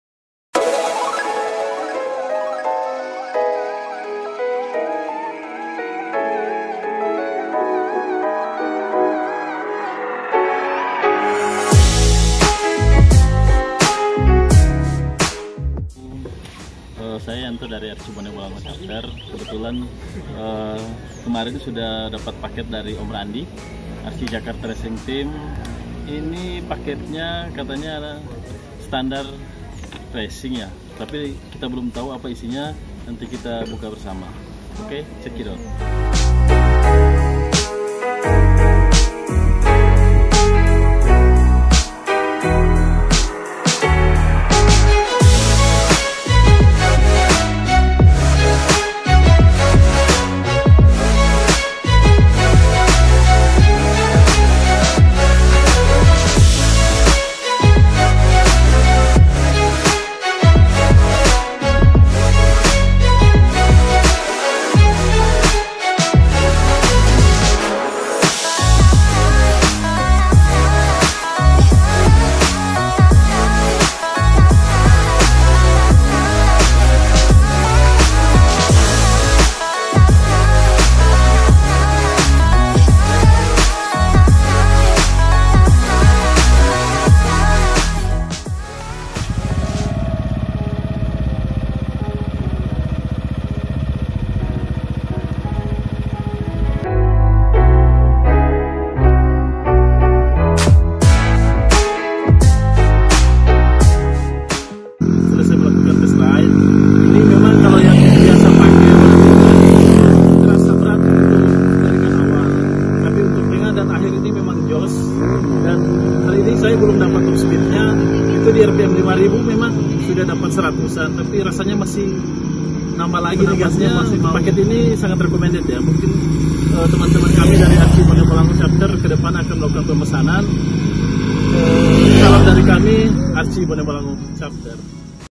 UPGRADE PERFORMA AEROX 155 VVA
Obat Ganteng yang bikin aerox 155 vva kamu yang standar jadi Josshhh... Yak ini dia SGO (Standar Gak Ada Obatnya). Produk yang merupakan hasil riset Arci Jakarta Racing Team yang bekerja sama dengan Sweet Martabak Racing yang melahirkan 3 komponen standar yang di rubah yaitu
per CVT,
per Kanvas Ganda dan
Roler.
Memang hanya 3 ini yg di rubah tp performanya awal hingga akhir berisi terus apalagi di RPM 5000 ke atas wadaw.

Jika ingin memesan produk SGO bisa langsung komen dan wa ke kami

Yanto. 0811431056